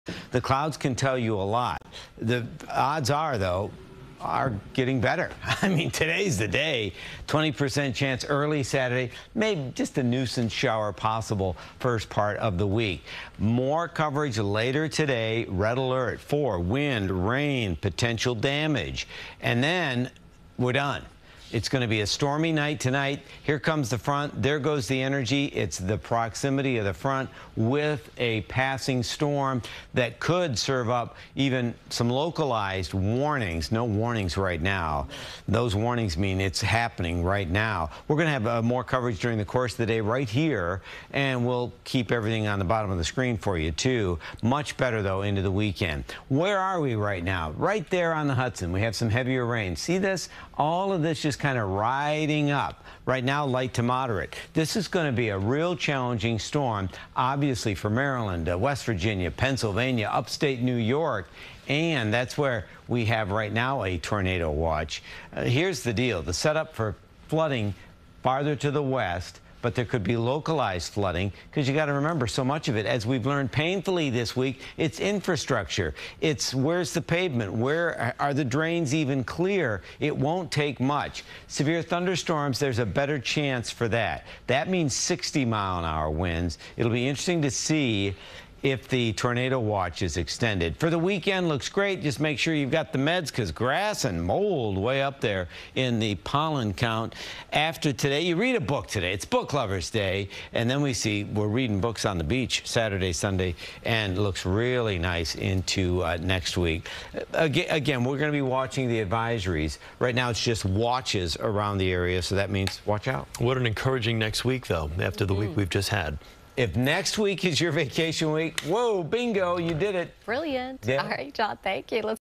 First Alert Weather: Remnants of Debby track farther west of NYC
CBS New York's John Elliott has the latest weather forecast as the remnants of Debby approach NYC and New Jersey.